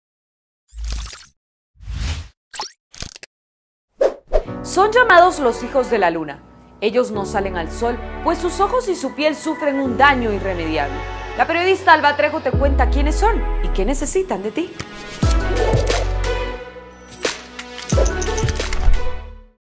Los niños de la Luna
Prefieren llamarse los niños de la Luna. Ya no salen al sol porque su piel y sus ojos sufren un daño irremediable. Conoce quiénes son y qué necesitan de ti.